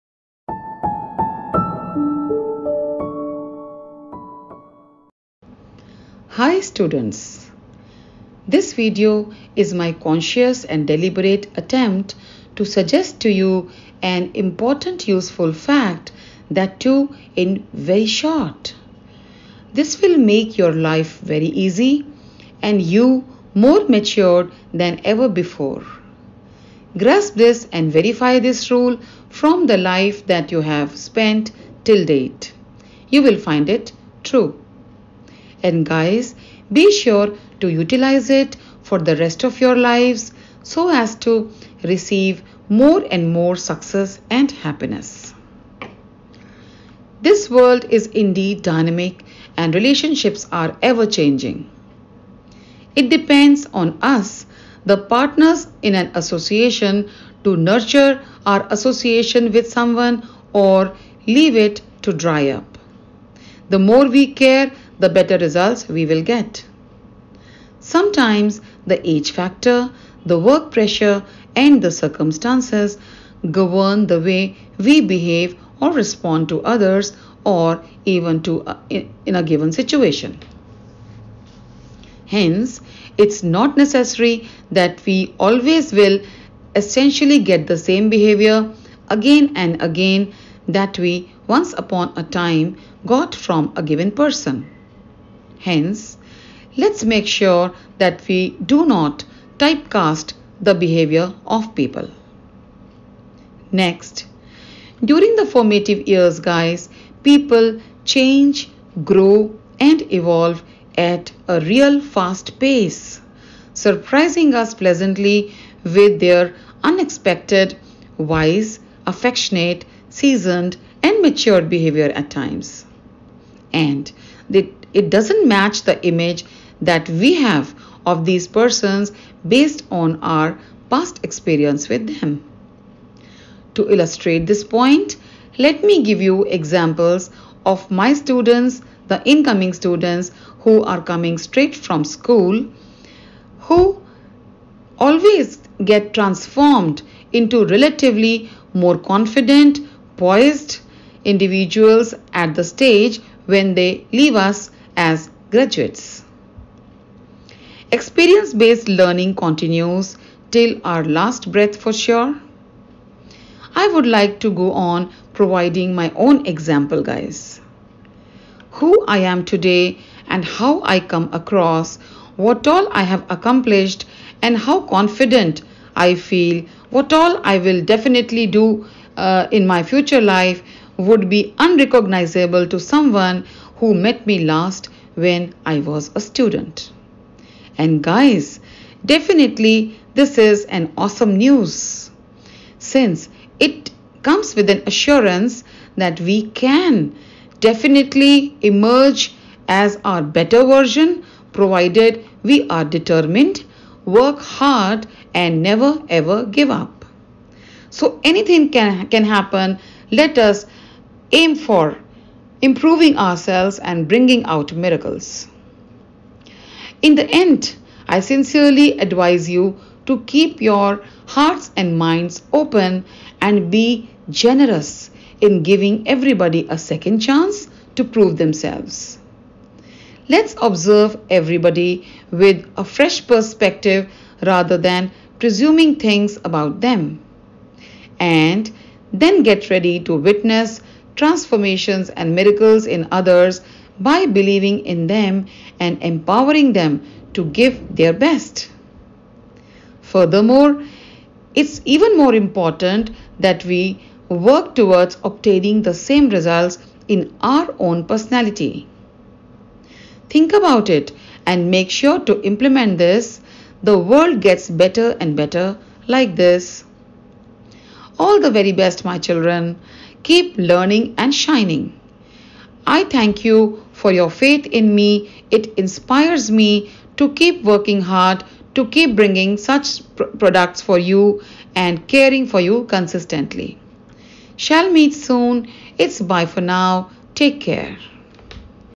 Keep your Minds & Hearts Open: The World is Dynamic & Ever Changing 🦋🌼🌹
If the changed behaviour of a loved one flabbergasts you, this short video is a must watch for you to deal with the situation wisely!